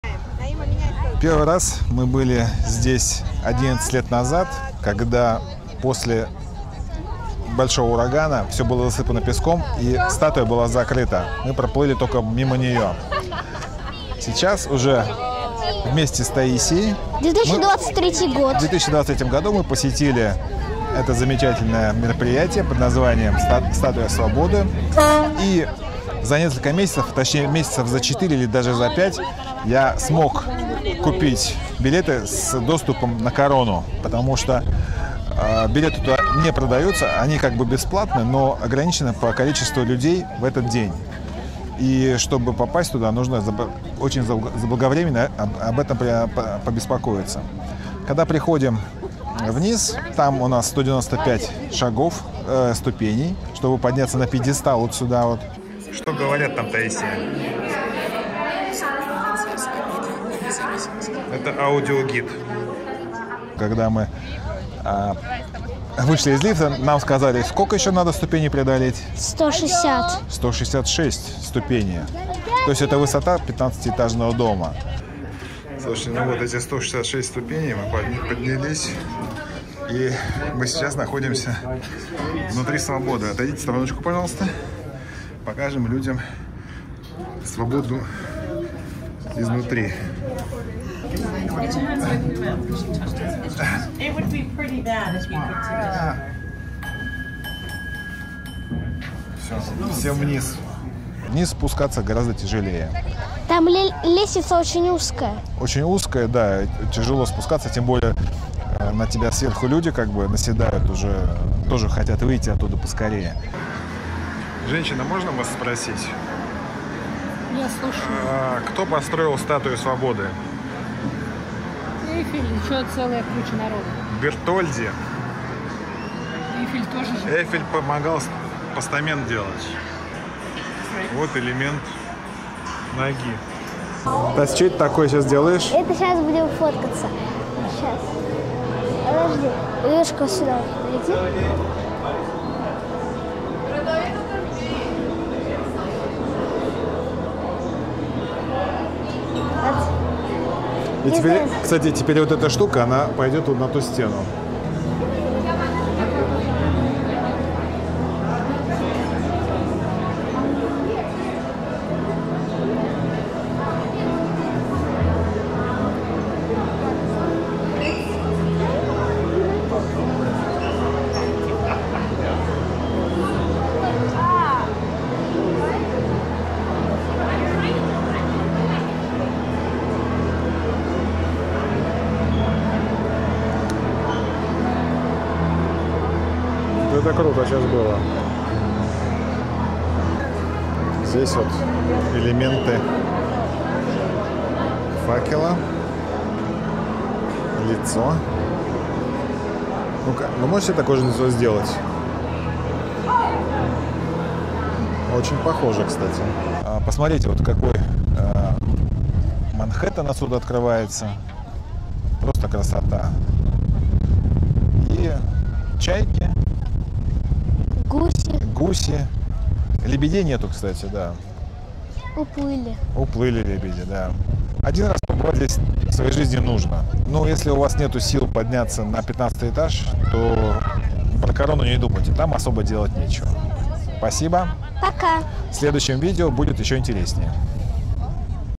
Что внутри статуи Свободы 🗽 Как туда попасть
1:03 ответ на вопрос в заголовке
Чтобы попасть внутрь статуи Свободы, которая находится на острове перед Нью-Йорком, нужно позаботиться об этом заранее.
Расскажем как это сделать и покажем что находится внутри самого известного монумента США. Так же поведаем нашу историю посещения этого памятника.